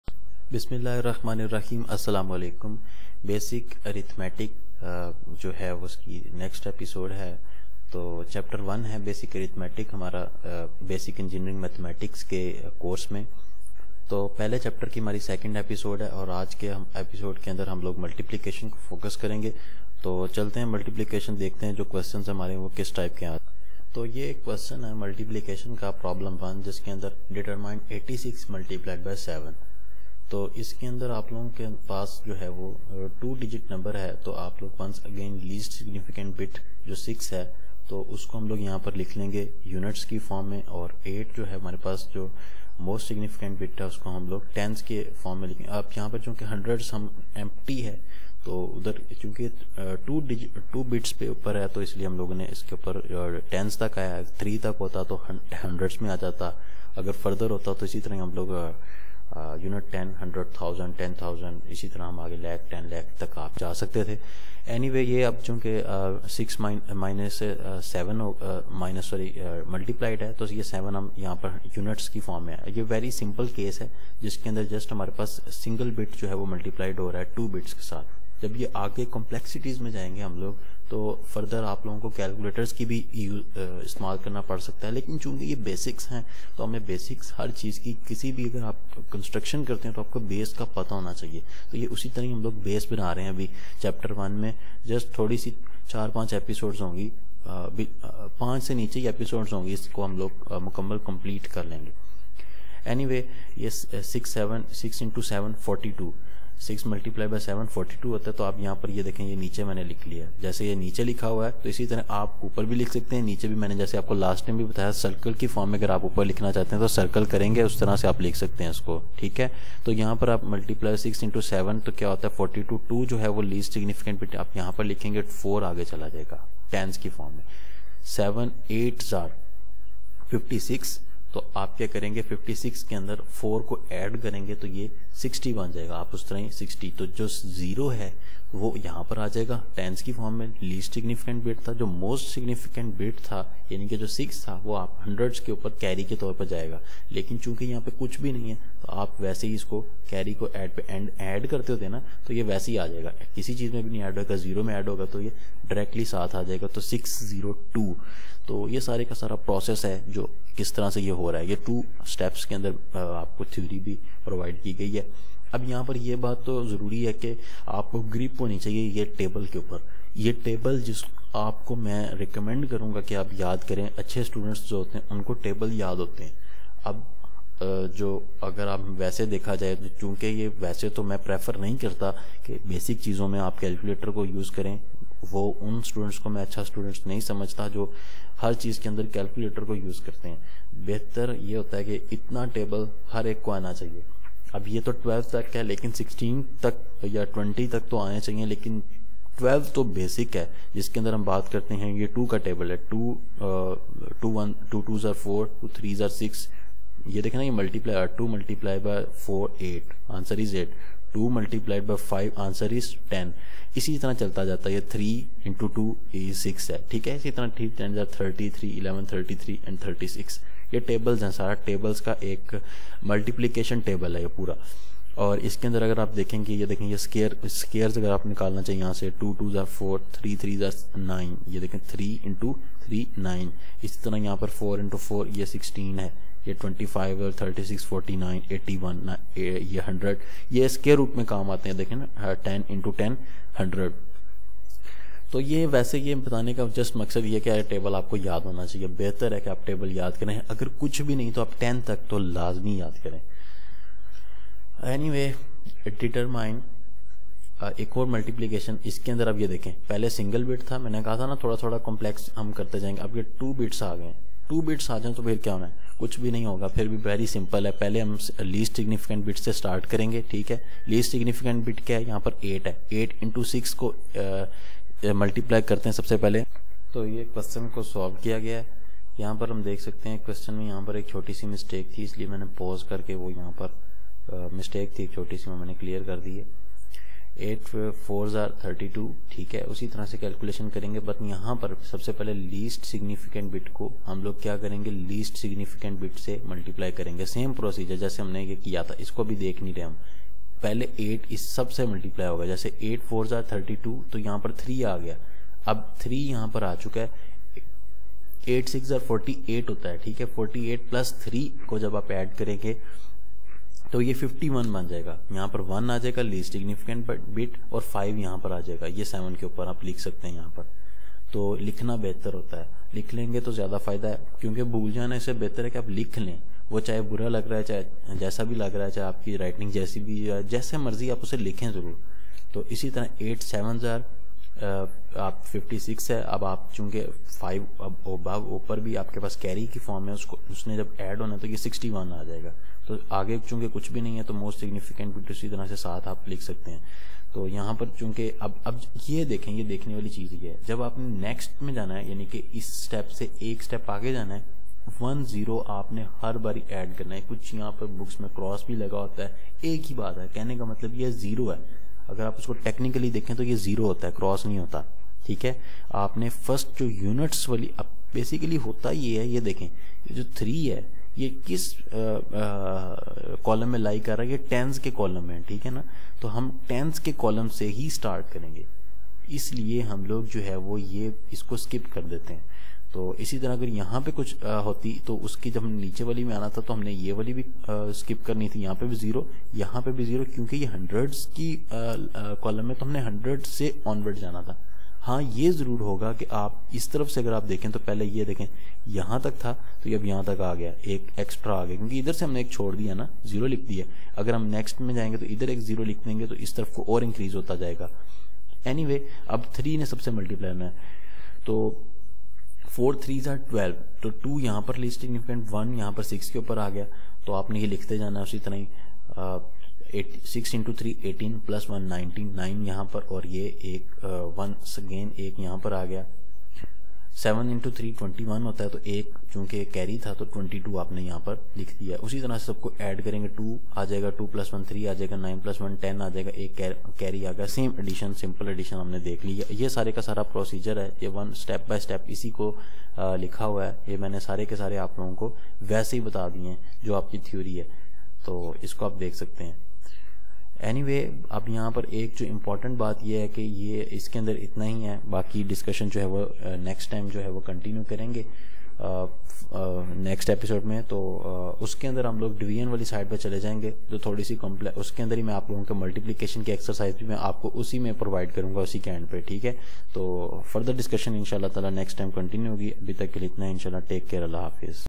Basic Arithmetic Episode 2 Urdu/Hindi
In this episode multiplication is explained .

Facebook.com/EasyLearn11